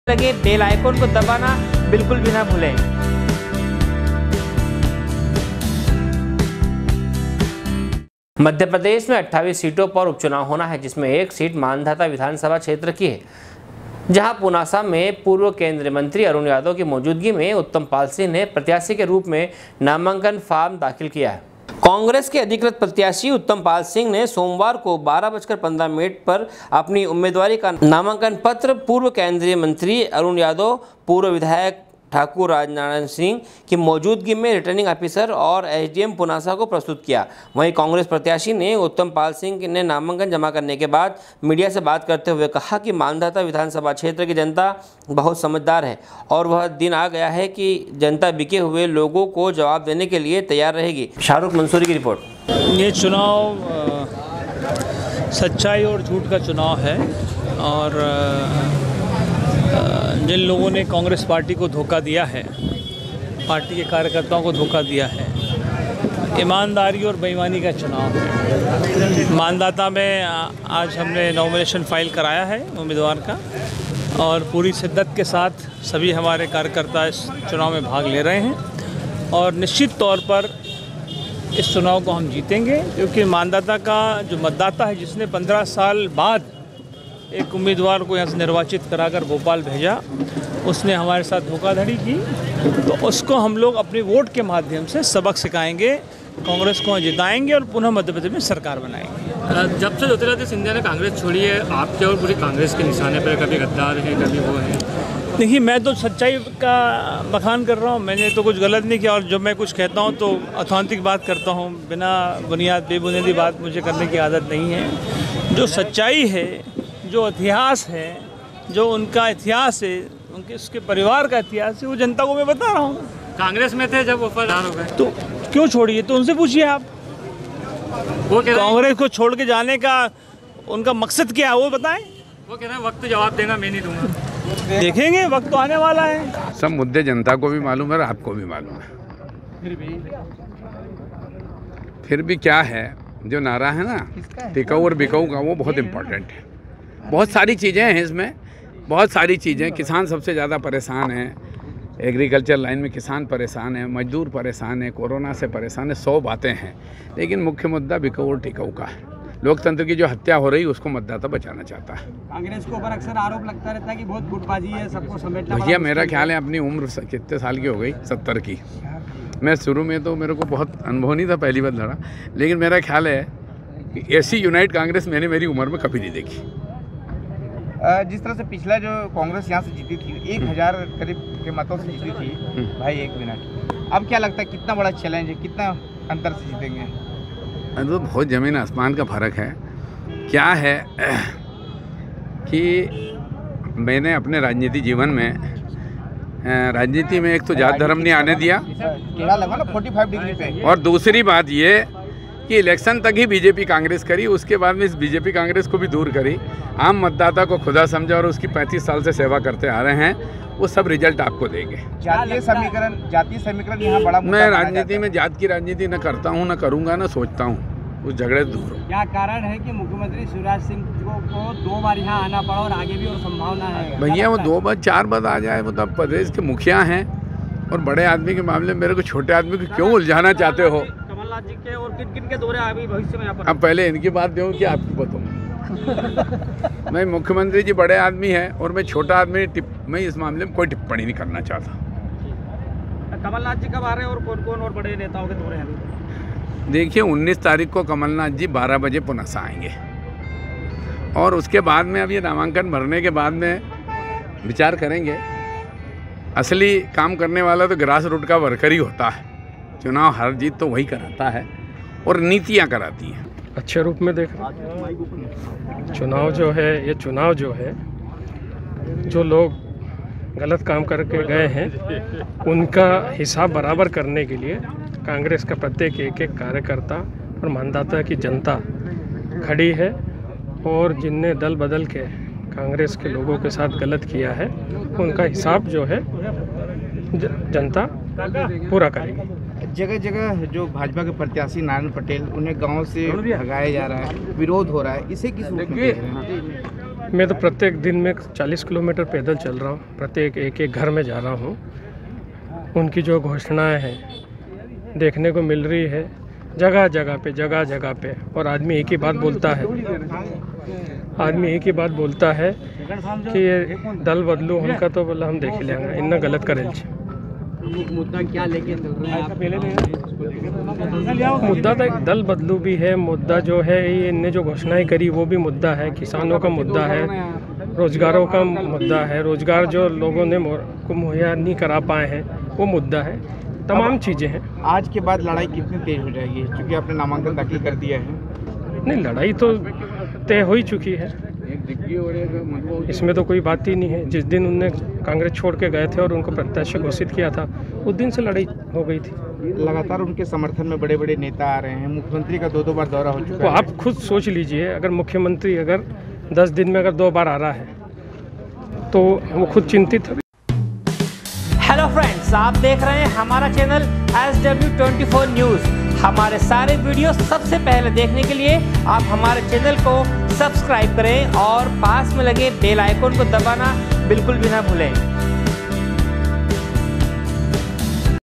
SW 24 News Madhyapradesh
मध्यप्रदेश में 28 सीटों पर उपचुनाव होना है जिसमें एक सीट मांधाता विधानसभा क्षेत्र की है जहाँ पुनासा में पूर्व केंद्र मंत्री अरुण यादव की मौजूदगी में उत्तम पाल सिंह ने प्रत्याशी के रूप में नामांकन फार्म दाखिल किया। मुंदी से शाहरुख मंसुरी की रिपोर्ट